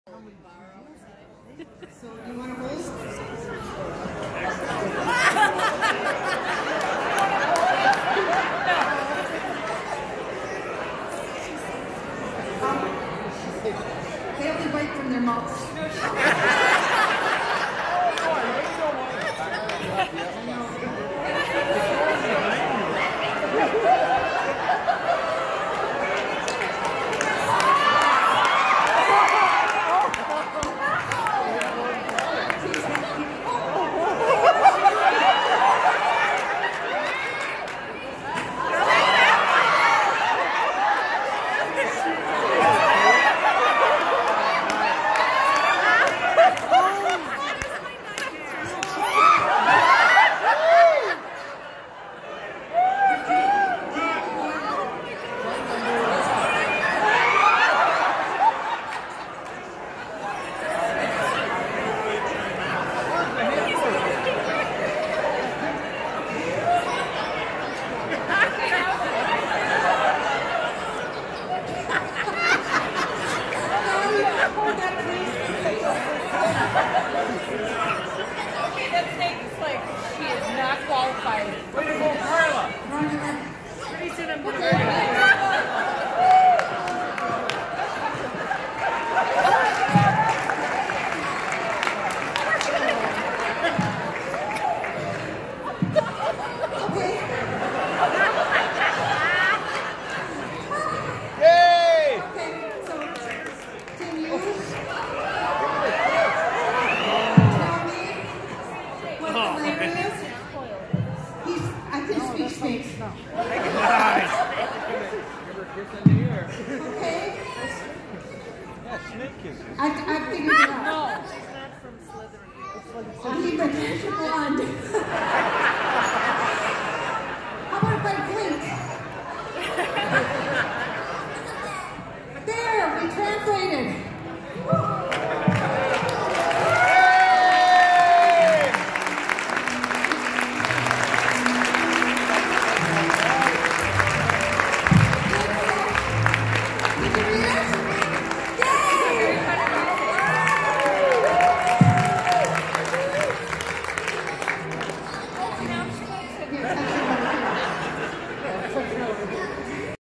Karla & a Boa Constrictor
Wishes & More Volunteer President, Karla Blomberg, uses a snake to let a wish recipient who is a big reptile fan know that her wish is coming true. You never know what's going to happen at the Wishes & More Winter Ball!